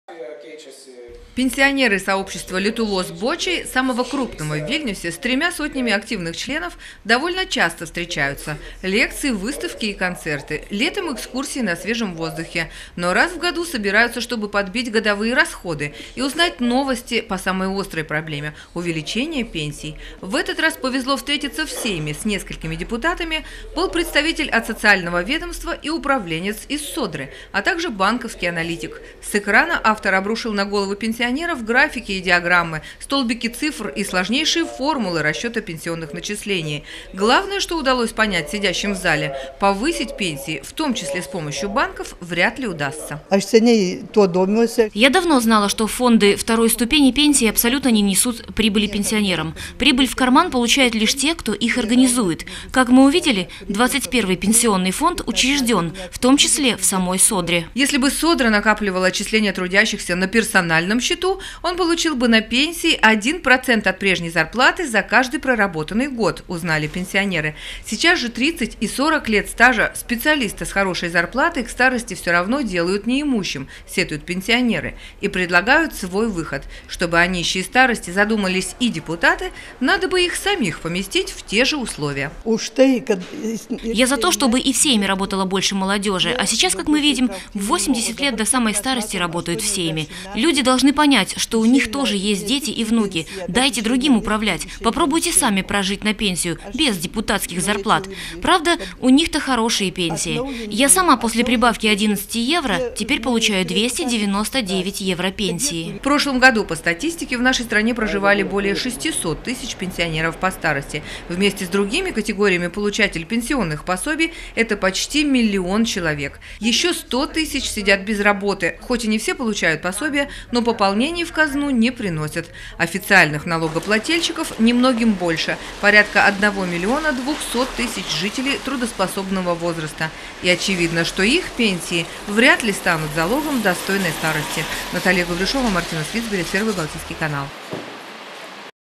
Вместо прибавки к пенсии – высшая математика!
«Взять под контроль последствия старения населения в стране» - под этим лозунгом сегодня в Сейме собрались сообщества пенсионеров. Однако, выяснилось, что советов и помощи от пожилых людей никто не ждал – их пригласили, чтобы ознакомить с формулами расчета пенсионных выплат.